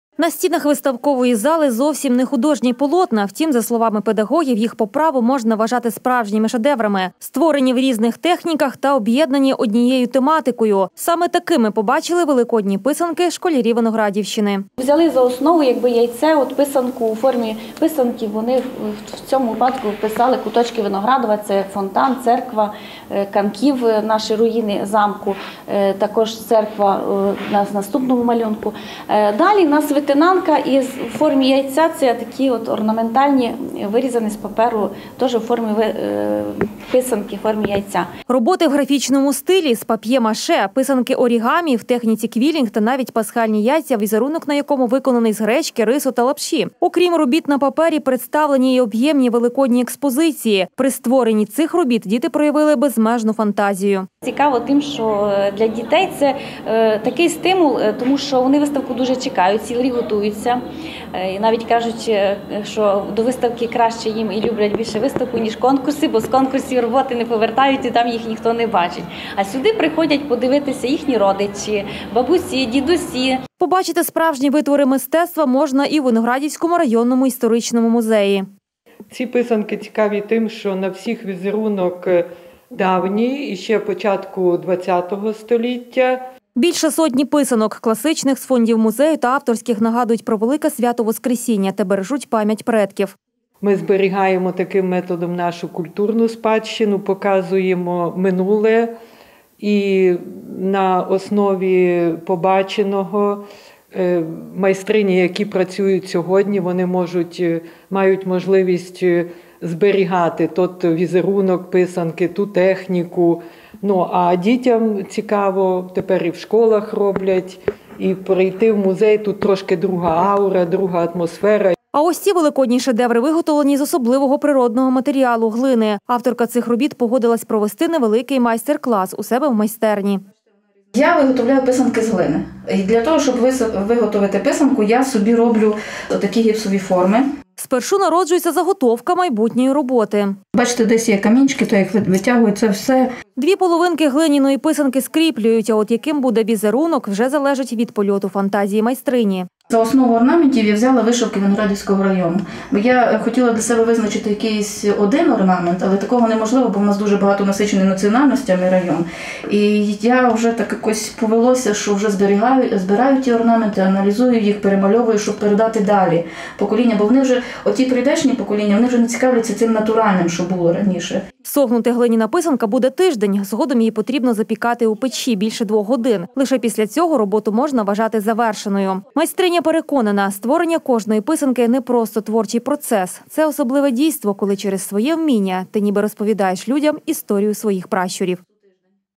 Унікальні писанки у Виноградові
Не просто символ свята, а справжній витвір мистецтва. Невід'ємний атрибут Великодня – крашанки вже давно побачити можна лише на святкових столах, а й на різноманітних виставках та у музеях. Зокрема й у виставковій залі у Виноградові. Створені в різних техніках, та об’єднані однією тематикою. Саме такими побачили Великодні писанки школярі Виноградівщини. Роботи в графічному стилі, з пап’є маше, писанки- орігамі, в техніці квілінг та навіть Пасхальні яйця, візерунок на якому виконаний з гречки, рису та лапші. Окрім робіт на папері, представлені і об’ємні великодні експозиції. Побачити справжні витвори мистецтва можна і у виноградівському районному історичному музеї.